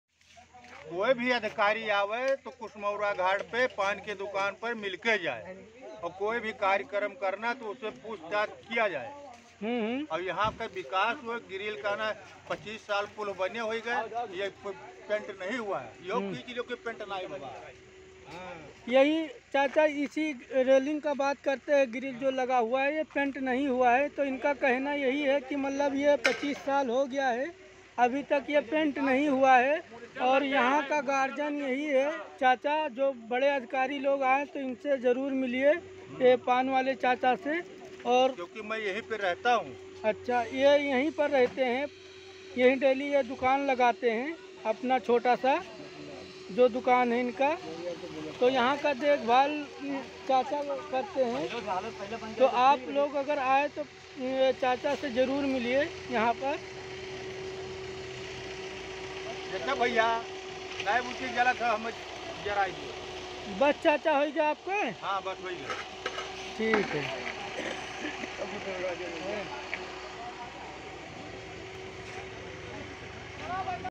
कुशमवरा घाट 25 साल हो गया पुल बने हुए जो रेलिंग लगा है अभी तक पेंटिंग नहीं हुआ है यहां का
MaddoBheekh sadullah Nagar